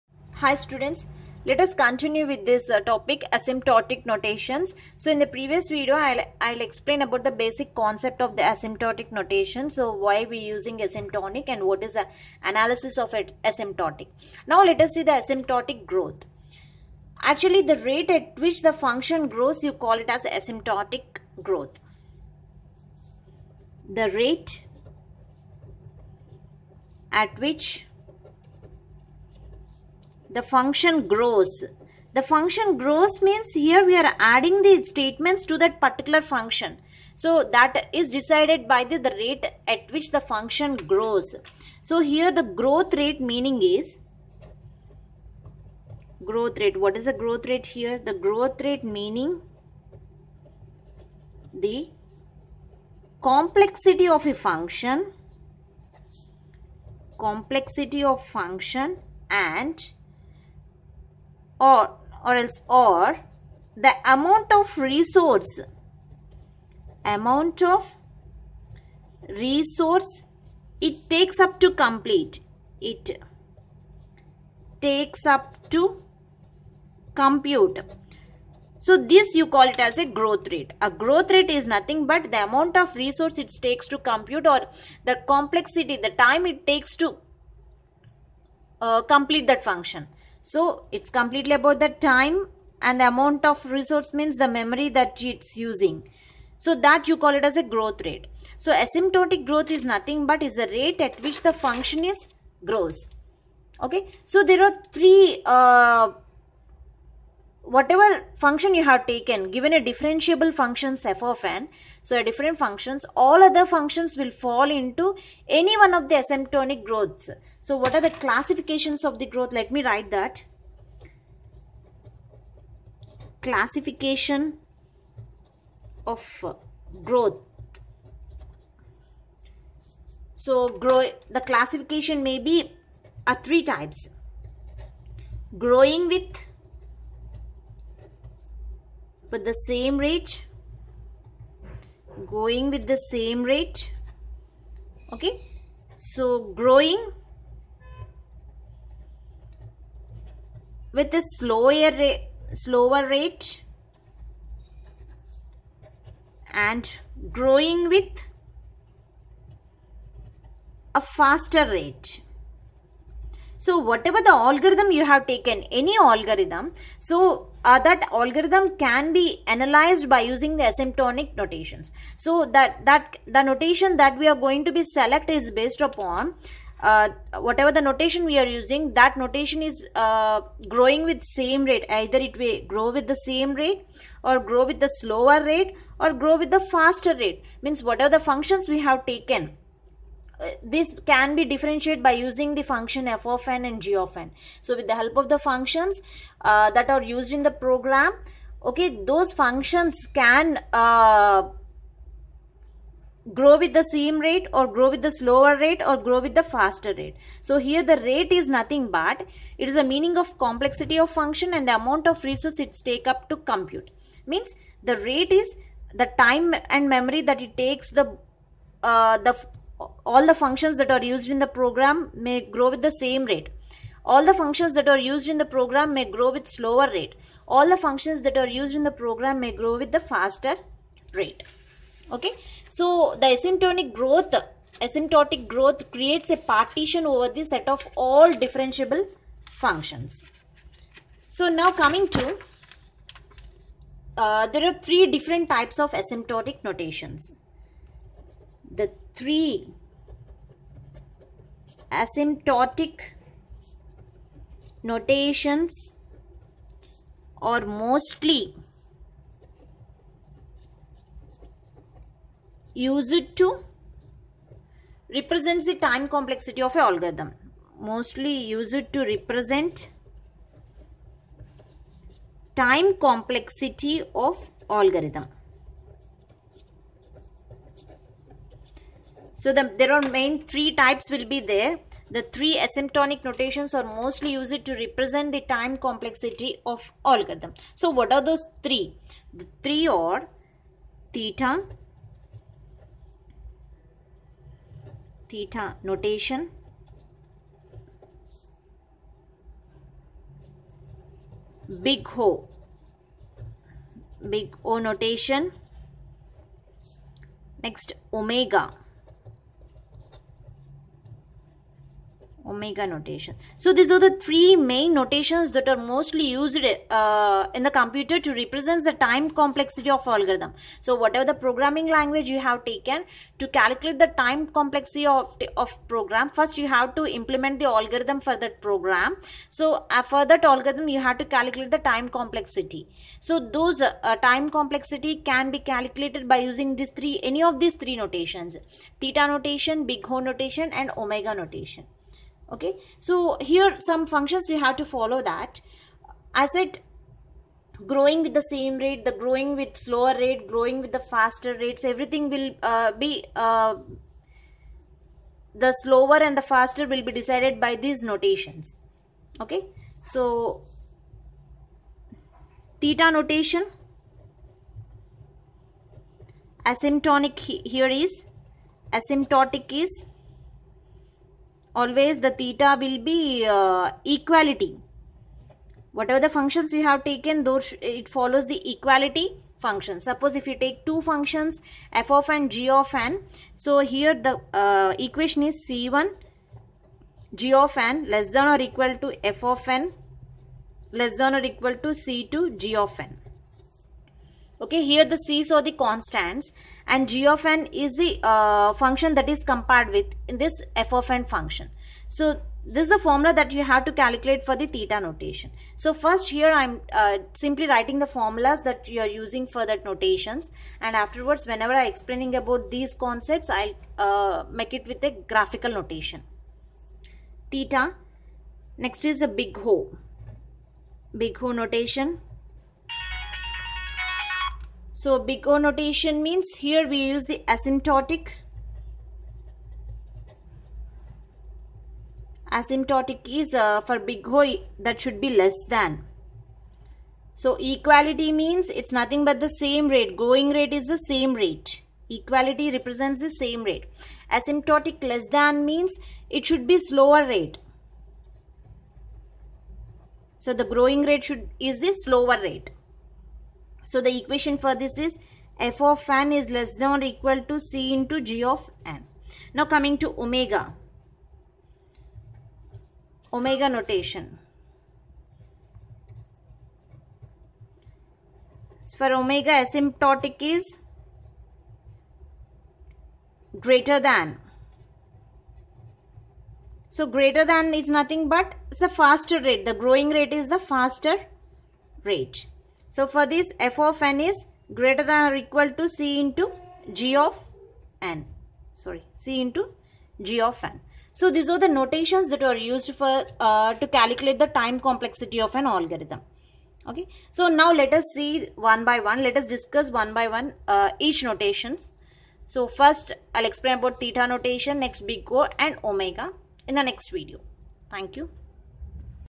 Asymptotic Growth Introduction In Computer Science | Design & Analysis of Algorithm (DAA) | Lec-7
This video discusses Computer Science Most Important and Interesting Section Design and Analysis of Algorithm (DAA) in detail with easy and detailed explanation.